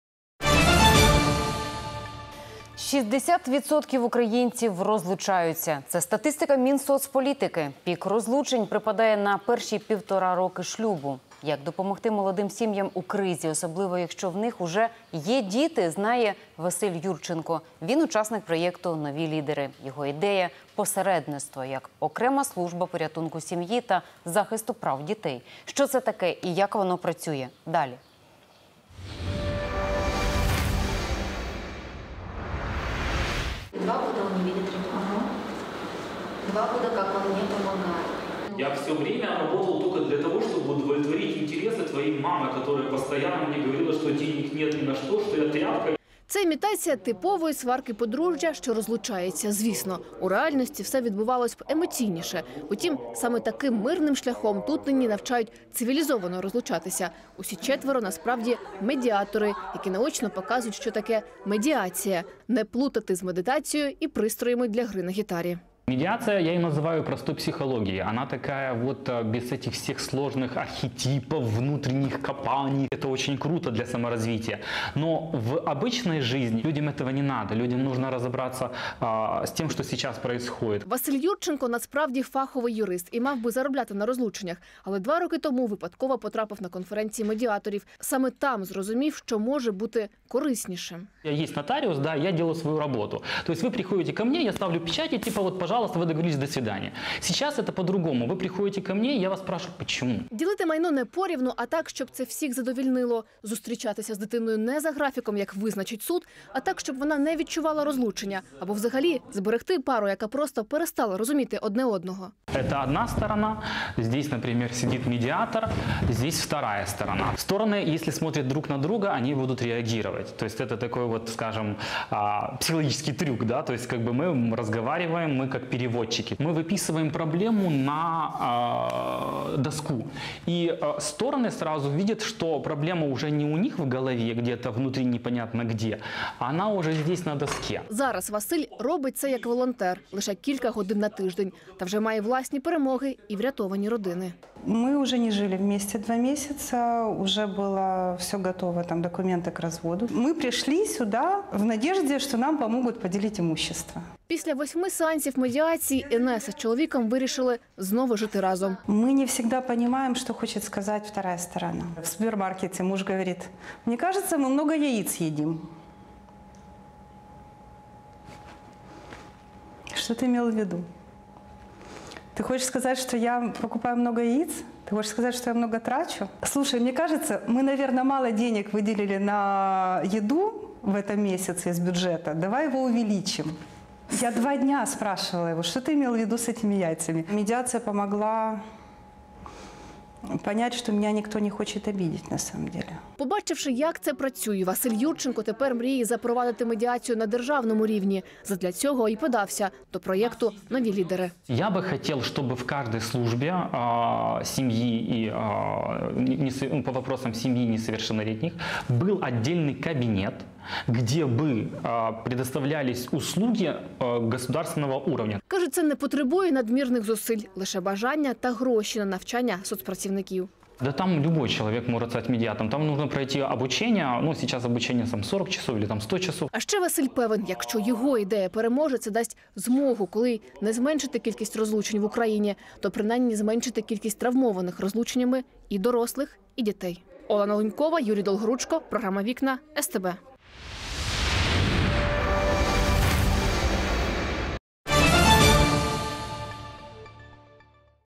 Помочь молодым семьям в кризисе - проект от участника Новых лидеров | Вікна-Новини
По статистике Минсоцполитики, 60% украинцев разводятся. Пик разводов приходится на первые полтора года брака.

Как помочь молодым семьям в кризисе, особенно если у них уже есть дети, знает участник проекта Новые лидеры Василий Юрченко.

Его идея - посредничество как отдельная служба спасения семьи и защиты прав детей.